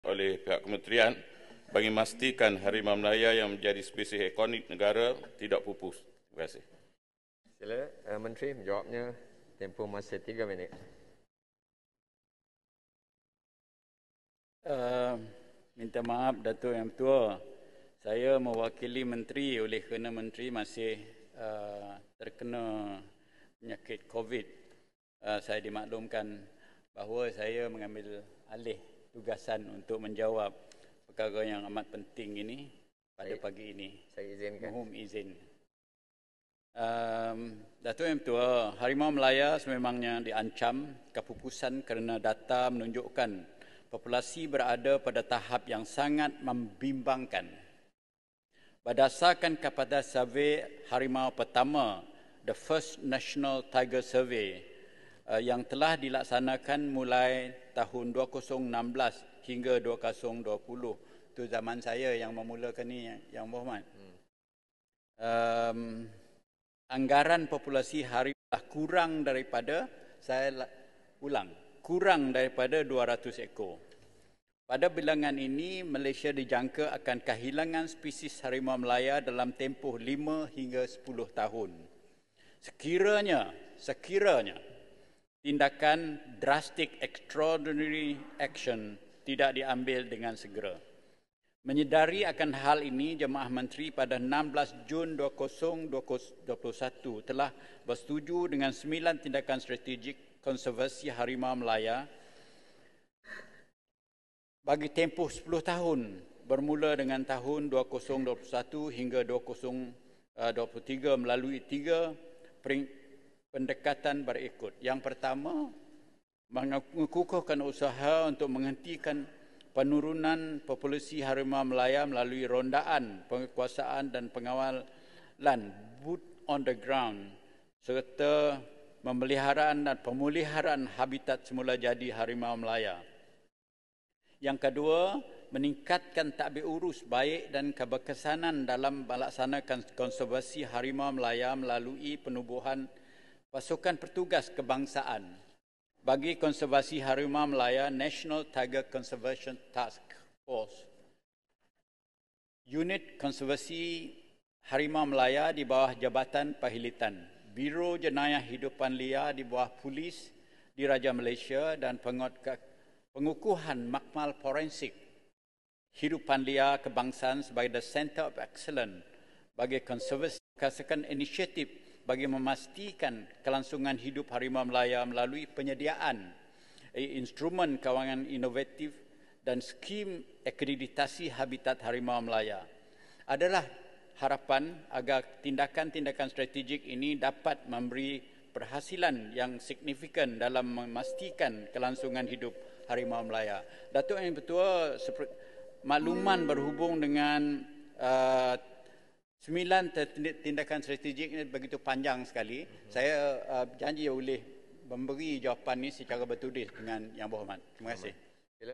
Jumlah Harimau Malaya kurang 200 ekor
Spesies Harimau Malaya atau harimau belang dijangka pupus dalam tempoh lima hingga 10 tahun jika tindakan drastik tidak diambil dengan segera mengatasi kepupusan haiwan itu.

Menteri di Jabatan Perdana Menteri (Parlimen dan Undang-undang), Datuk Seri Wan Junaidi Tuanku Jaafar berkata, Harimau Malaya sememangnya diancam kepupusan kerana data menunjukkan populasinya berada pada tahap yang sangat membimbangkan.